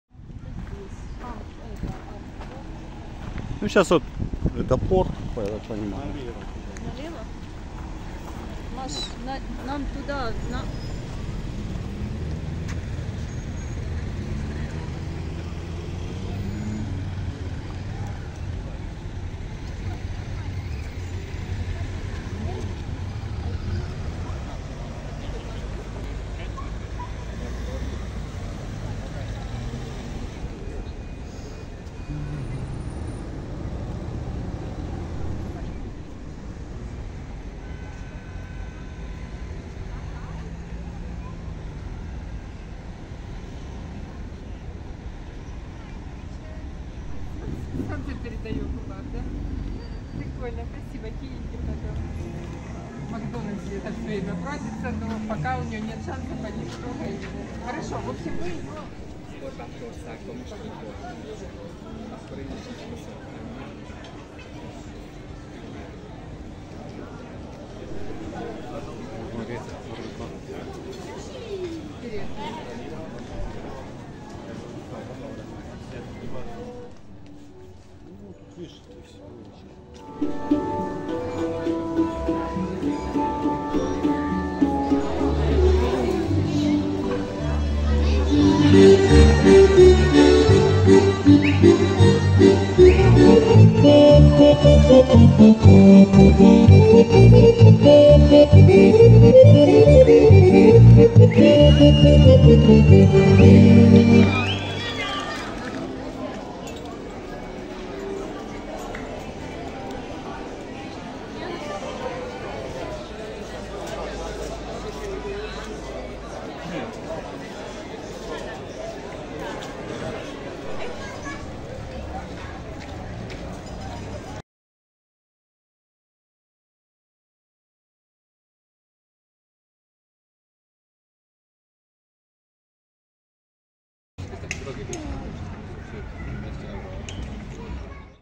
Вечірня прогулянка містом Пула, Хорватія (культурна пам'ятка біля Turisticko naselje Duga Uvala)
Автор відео: Стегній Павло (блог - практичний психолог, психотерапевт, майстер системного моделювання та розстановок. Маю ступінь доктора философіїї (кандидат історичних наук), доцент.
Це відео про те, що можна подивитися, якщо ви відпочиваєте в Turisticko naselje Duga Uvala або десь поряд. Відносно недалеко знаходиться місто Пула, куди бажано поїхати хоча б один раз на кілька годин.
Пула (хорв. Pula, італ. Pola) — місто і порт на узбережжі Адріатичного моря, найбільше місто в південній частині півострова Істрія. З часів Стародавнього Риму Пула також була адміністративним центром римської провінції Істрія. Тому тут є кілька історичних пам'яток переважно античних часів. Це, перш за все, амфітеатр ), Катедральний собор Внебовзяття Пресвятої Діви Марії (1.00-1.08), форум (центральна частина міста), храм Августа (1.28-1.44), арка, що колись була воротами в місто (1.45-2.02).